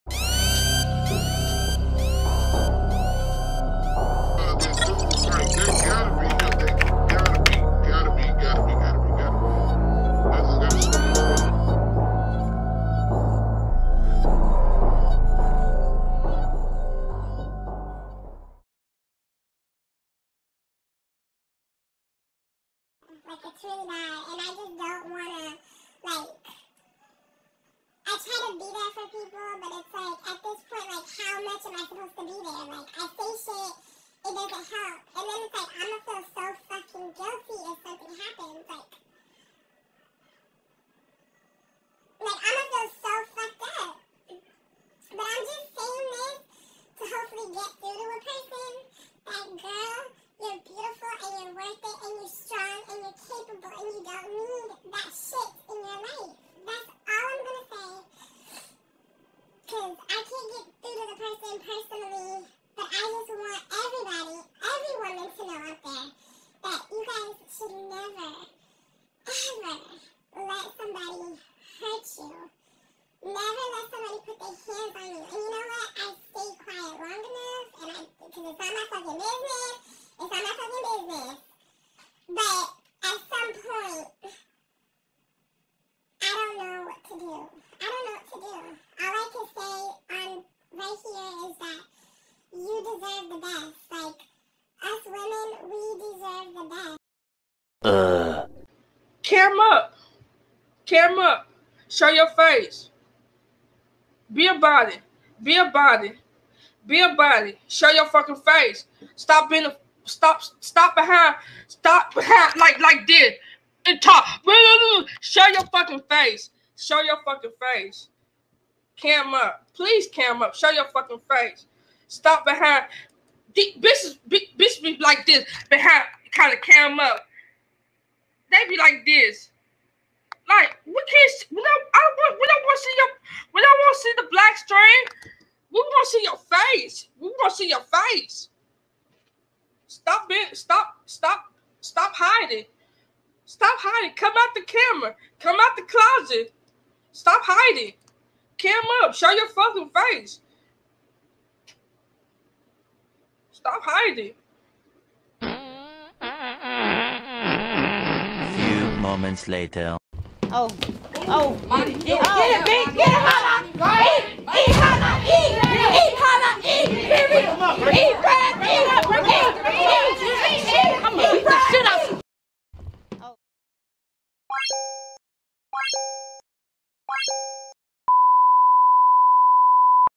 Jasmine's Friend Confirms Abuse Again / Yung Melo Speaks On ‎@MyJazzyLife /
@MyJazzyLife @extrajazzchris4657   Jasmine's best friend confirms watching and hearing multiple beatings from Chris Hips.../ Yung Melo addresses Jasmine one on one! / More to come!